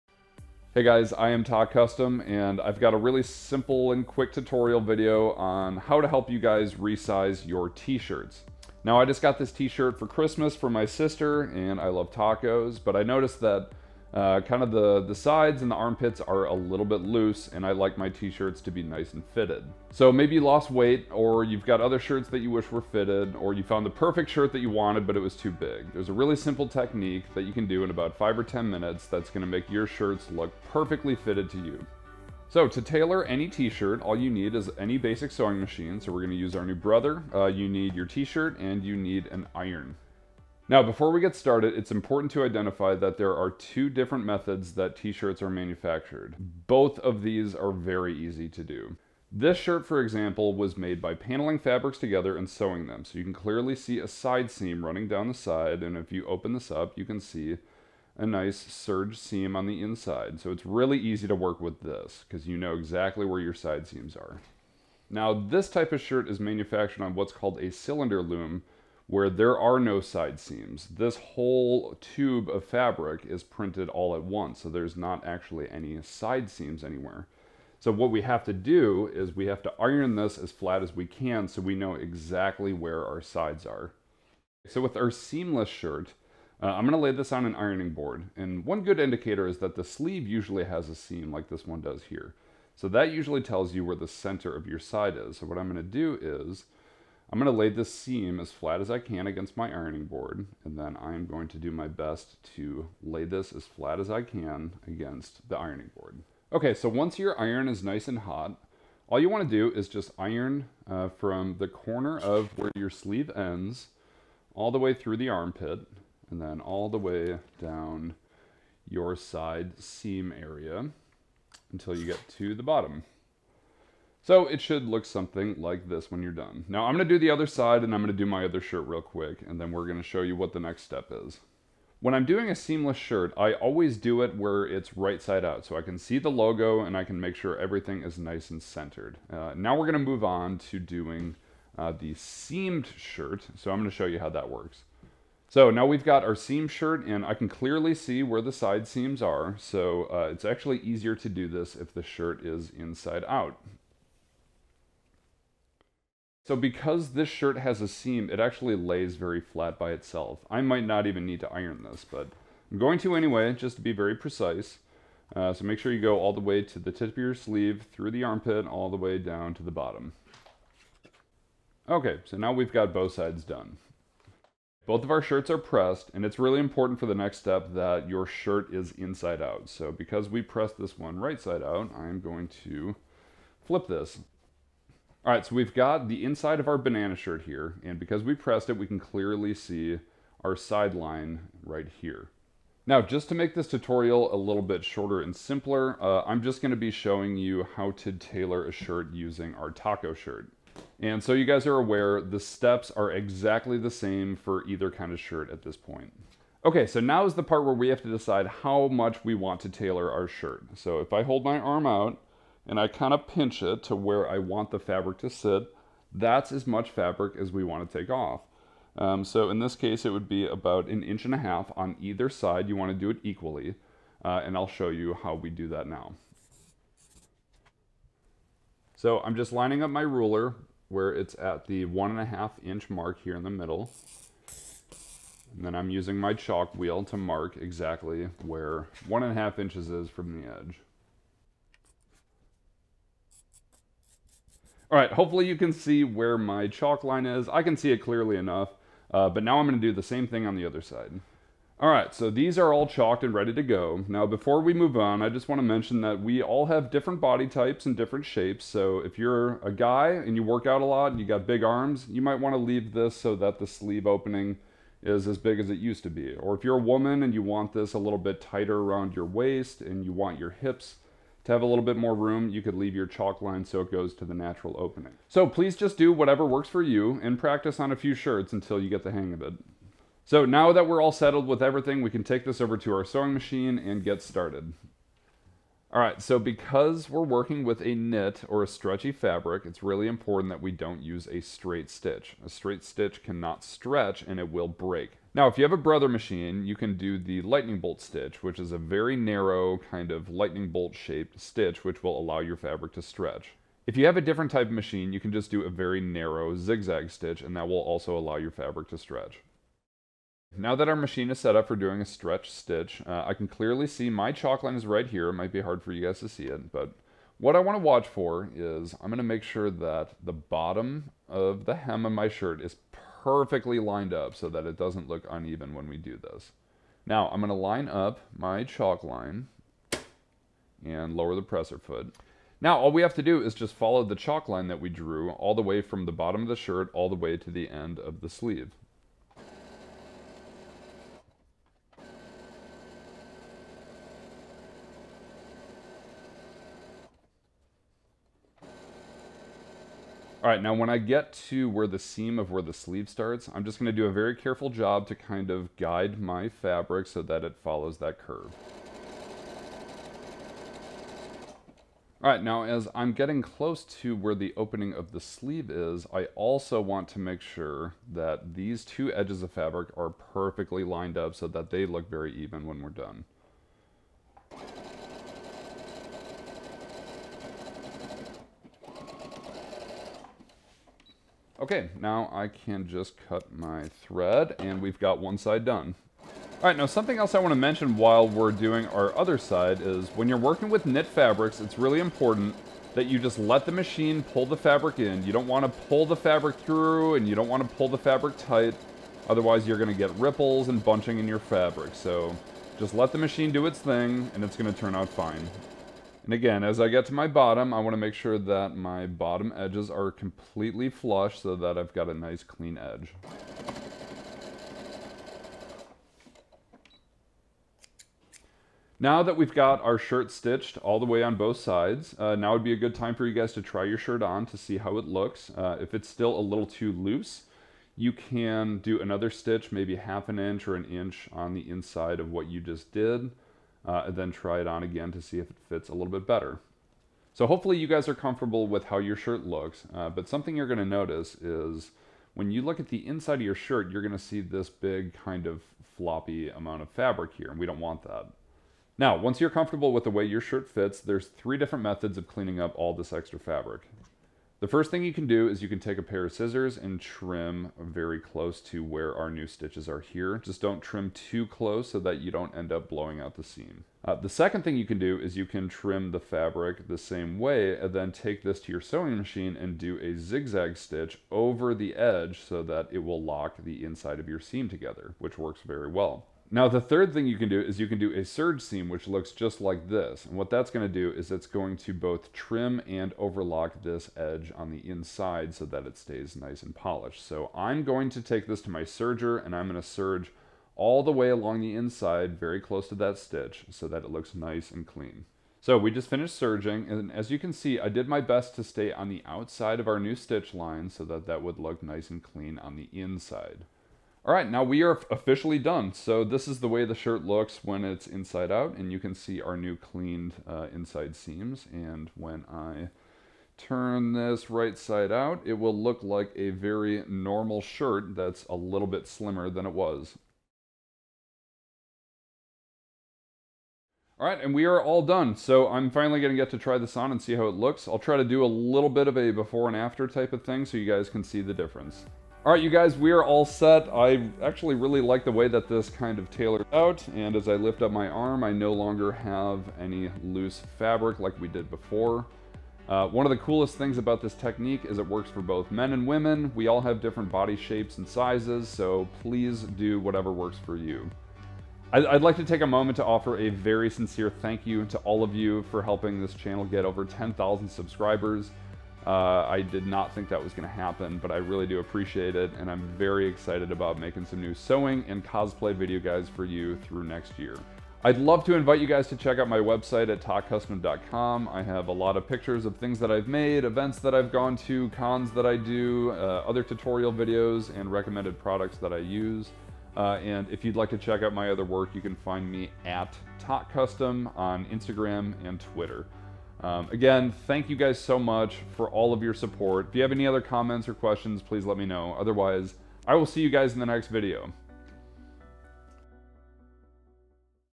How to make a t-shirt fit perfectly - Beginner level sewing technique! How to use a sewing machine
In this video, I show a very simple trick that can turn a T-shirt, into a fitted and tailored shirt that fits you perfectly!

and you'll need a T-Shirt, and Iron, and maybe some chalk or a pencil.

For high quality fabric scissors and cutting tools, take a look at Famore Cutlery